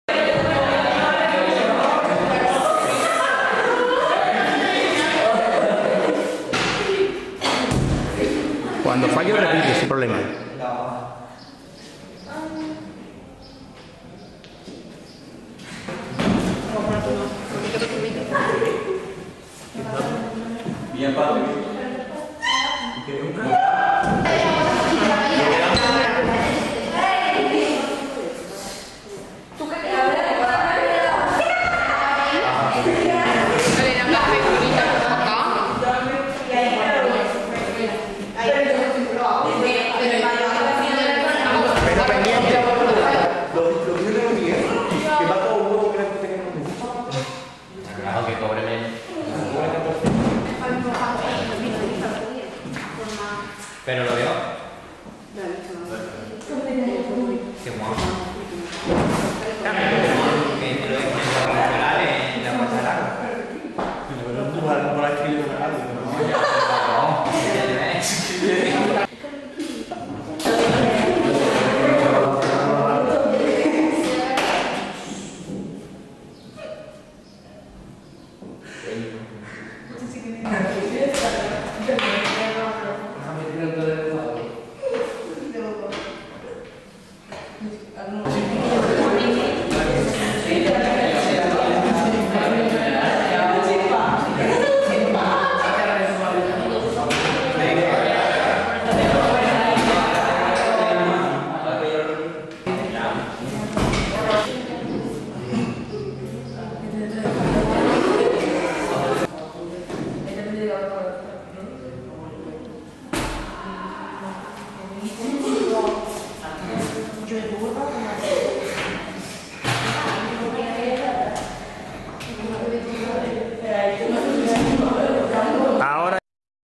2012/13 Equilibrio invertido
Curso 2012/13, están en 3º ESO y en el IES Olontigi, Ezequiel, Alvaro, Patricia, Cristian P., Jesús A., Rosana, David, Manu, Alejandro, Razvan, Pedro, Lara, Cristian B., Pablo, Jesús,  y Cristian M. realizan el equilibrio invertido.